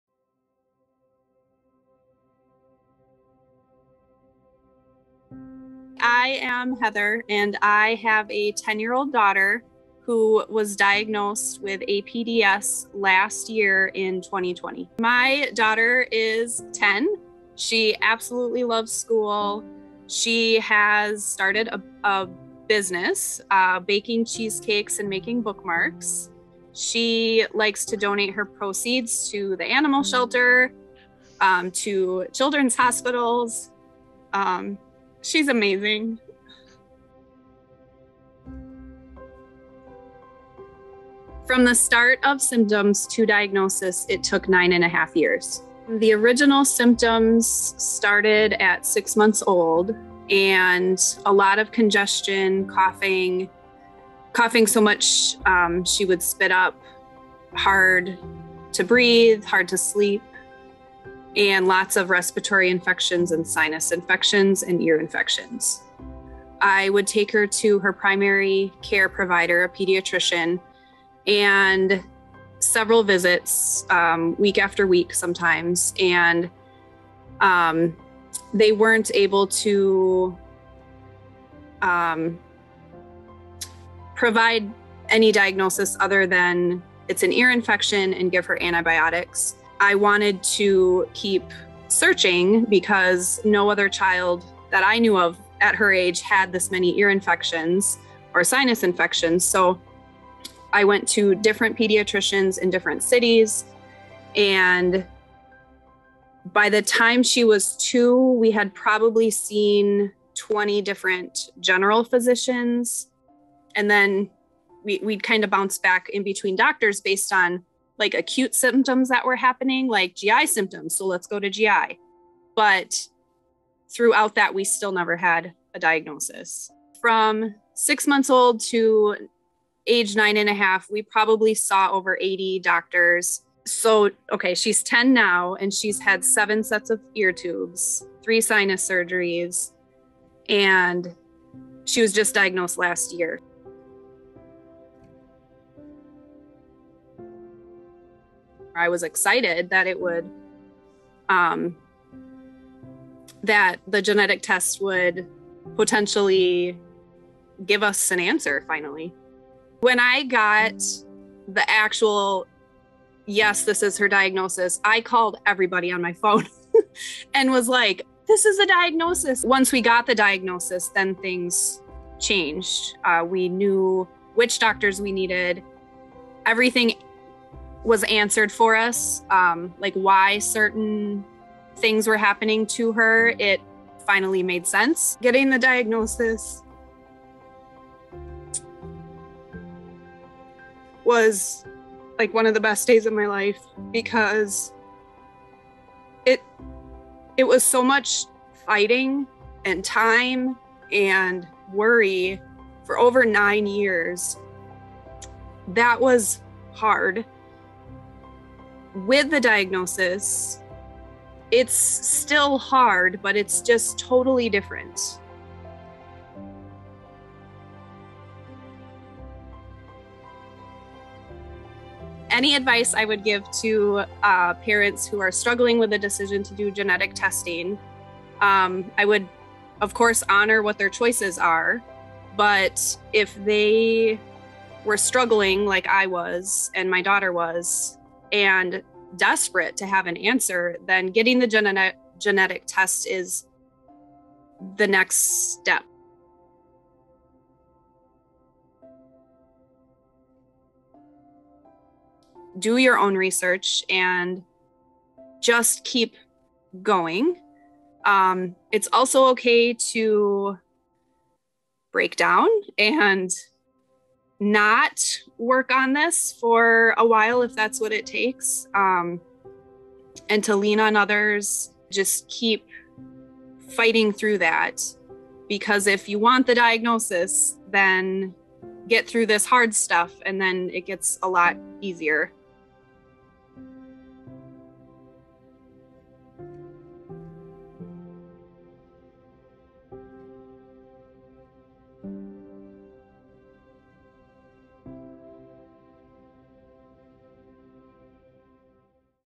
Heather's Story - A Mother's Journey with APDS
Heather is the mom of an amazing 10-year-old daughter who was diagnosed with the rare primary immunodeficiency, APDS.  This is her story.